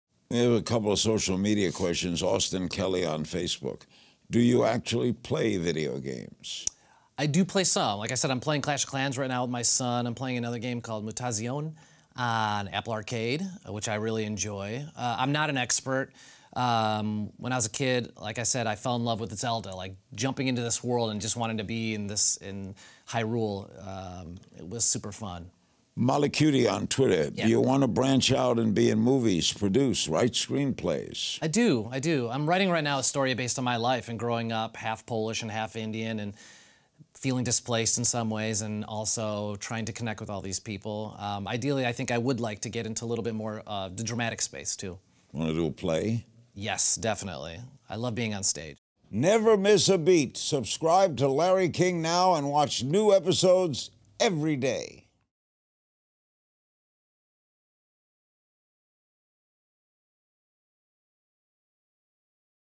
Future plans, video games, & directing -- Danny Pudi answers your social media questions
Danny Pudi reveals that he would love to produce and direct in addition to acting. Plus, he’s on a show about video games, but is he any good at playing them himself?